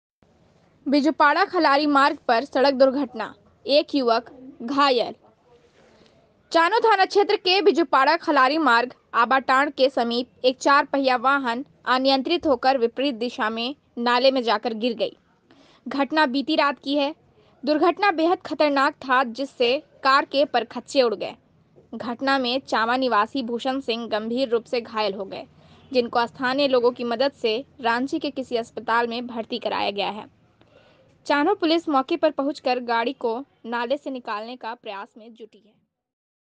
बीजूपाड़ा खलारी मार्ग पर सड़क दुर्घटना एक युवक घायल
चान्हो थाना क्षेत्र के बीजूपाड़ा खलारी मार्ग अंबाटांड के समीप एक चार पहिया वाहन अनियंत्रित होकर विपरीत दिशा में नाले में जाकर गिर गई घटना बीती रात की है  दुर्घटना बेहद खतरनाक था जिससे कार के परखचे उड़ गए घटना में चामा निवासी भूषण सिंह गंभीर रूप से घायल हो गए जिनको स्थानीय लोगों की मदद से राँची के किसी  अस्पताल में भर्ती कराया गया है।
चान्हो पुलिस मौके पर पहुंचकर गाड़ी को नाले से निकालने के प्रयास में जुटी है